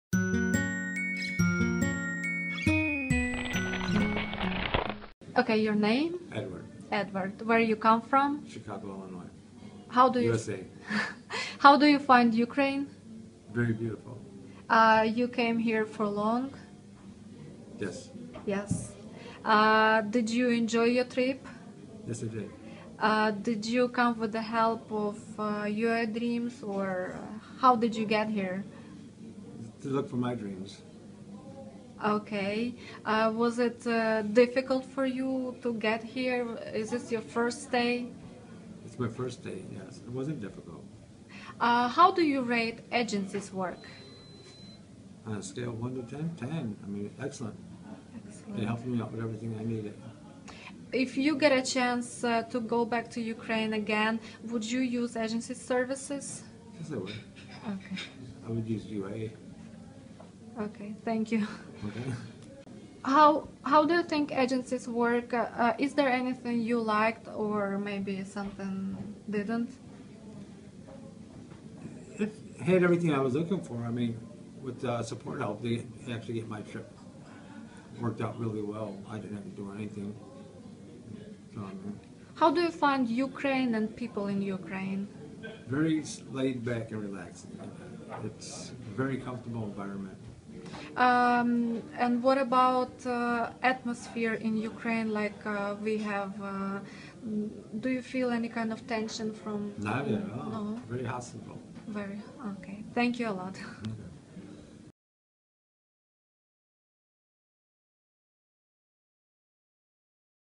Edward: They work with me every step of the way
I am very happy with the service that I received. They work with me every step of the way. The agency even felt bad that I had problems with my flight and they work with me every step of the way and the lady was less shy than me and my interpreter was absolutely wonderful. We found a nice place to eat and I thought the meeting went well with the lady and the city is amazing. I will come here again if the lady chooses to see me. I like to thank the agency for helping me see this dream come. True thank sincerely. Edward